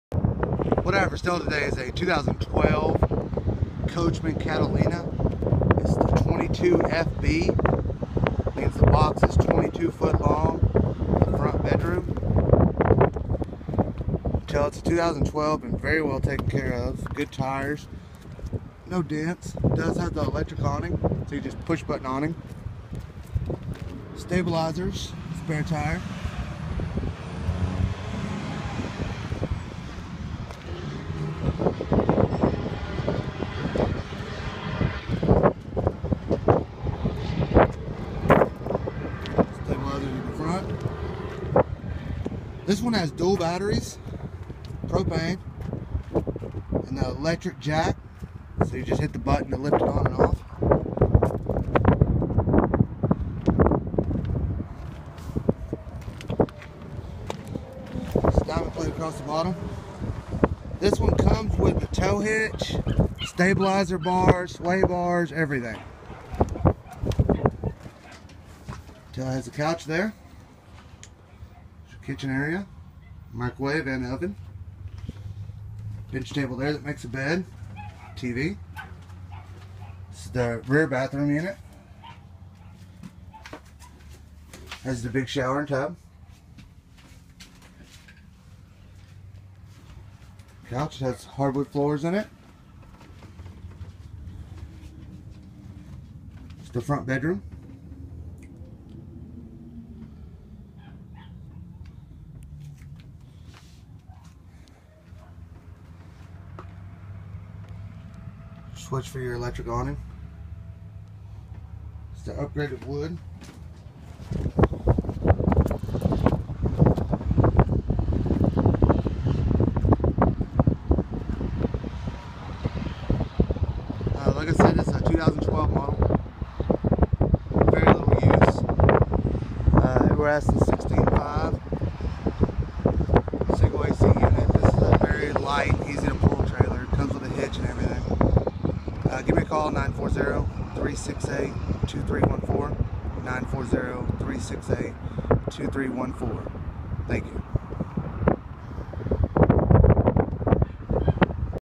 2012 COACHMEN CATALINA 22FB FOR SALE $12,750
2012 Coachmen Catalina 22fb for sale $12,750 ,

CHECK 287CONSIGNMENTS.COM TO SEE EVERYTHING WE HAVE FOR SALE BY OWNER.

Catalina by Coachmen with Rear Full Bath Including: Wardrobe, Sink with Medicine Cabinet, Toilet & 24x40 Full Tub, Refrigerator, 3 Burner Range & Microwave, Kitchen Sink, Booth Dinette, TV Entertainment Cabinet, Overhead Storage Throughout Unit, Sofa with Half Wall Behind Separating Bedroom from Living Area, Front Queen Bed, 2 Wardrobes Both Sides of Bed, Front Pass-thru Storage, and More! Full Wall Behind Sofa

Length 26'5"
Width 8'0"
Height 10'7"
Gross Weight 7,000 lbs
Cargo Weight 2,770 lbs
Furnace BTU 30,000
Sleeps 5
Tire Size 15"

Can be seen in haslet tx on the north side of ft. worth

Open to cash offers in person only...